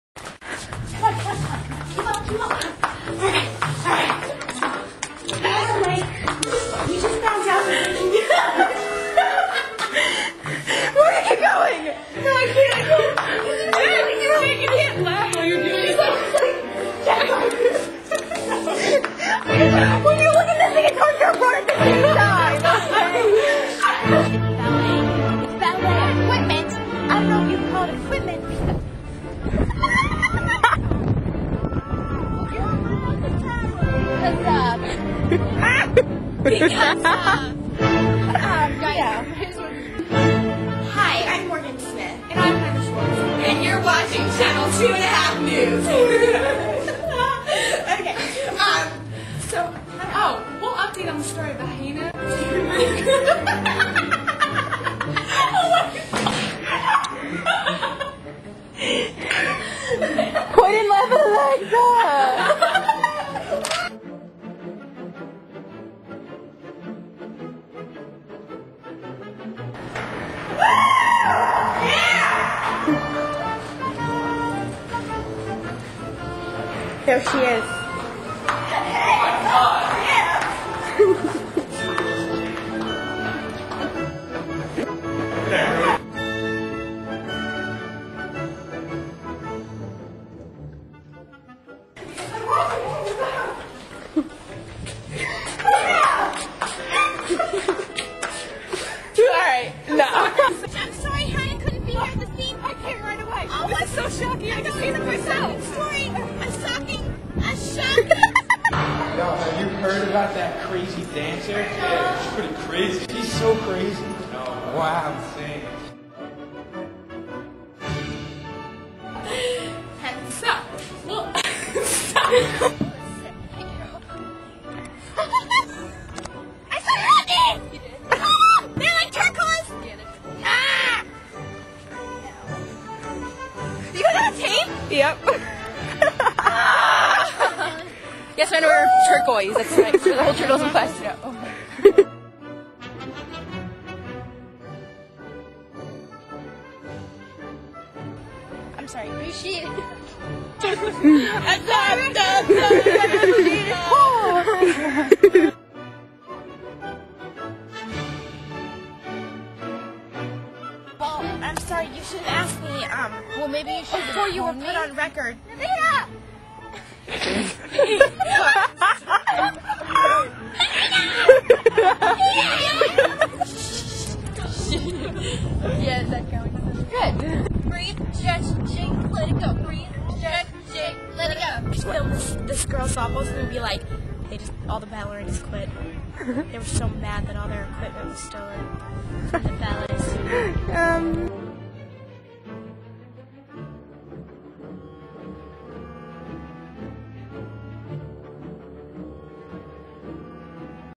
Bloopers
Bloopers from a journalism video made with Morgan, Jen and Alexa